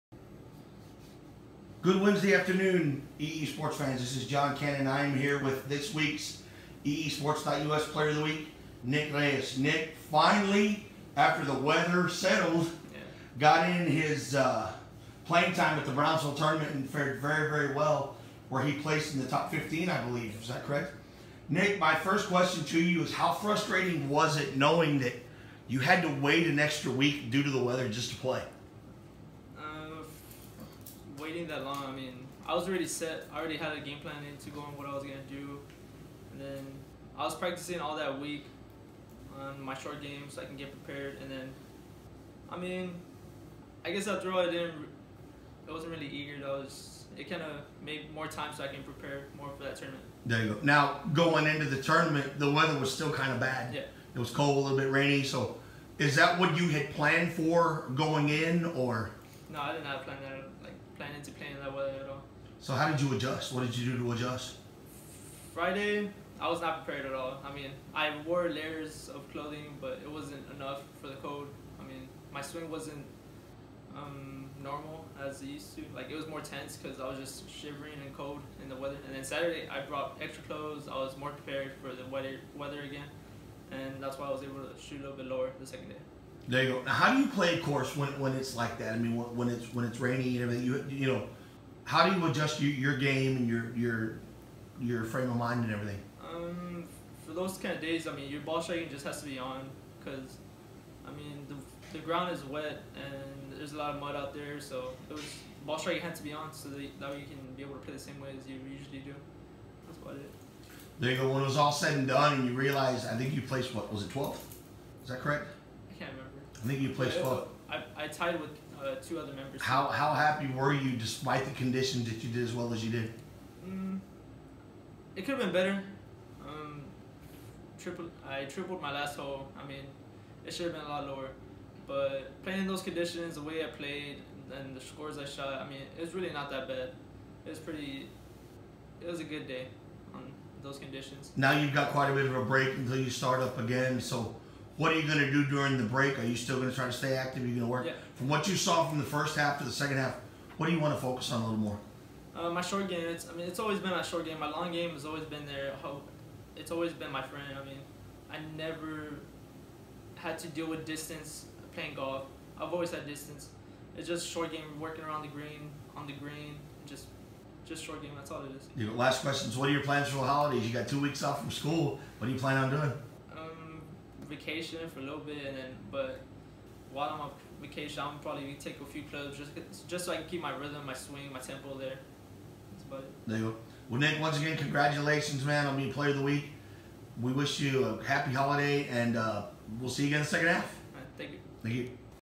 Nick Reyes POW 12-20
Boys golfer Nick Reyes has been selected as the EESports.us Player of the Week.